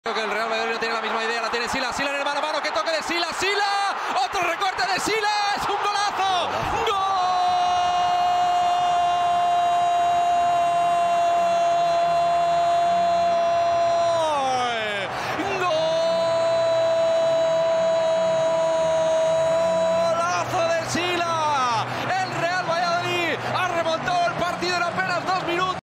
¡Goolazo de SYLLA 😮!
Espectacular gol del Real Valladolid frente al SD Eibar.